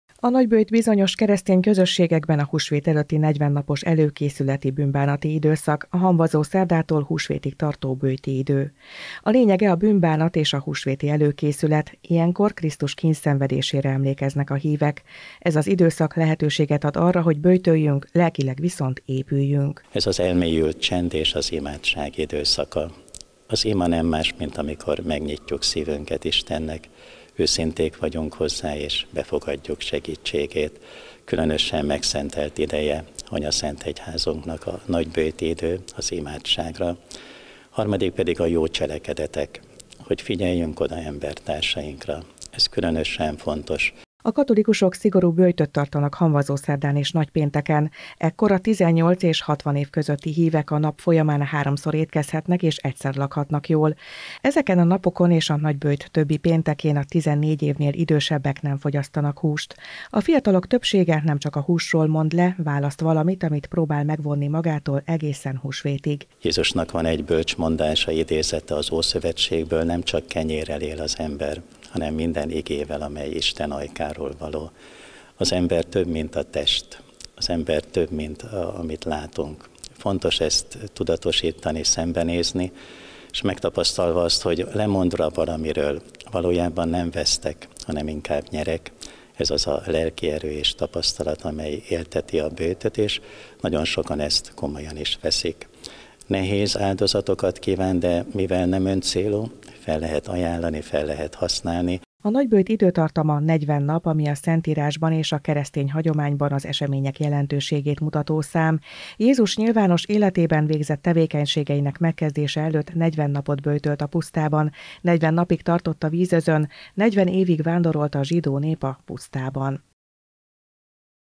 Elkezdődött a böjti időszak
Hamvazószerdával elkezdődött a nagyböjti időszak. A húsvét előtti negyven napos bűnbánati időben hamvazószerdán és nagypénteken szigorú böjtöt tartanak a hívők, és a péntekeken nem fogyasztanak húst.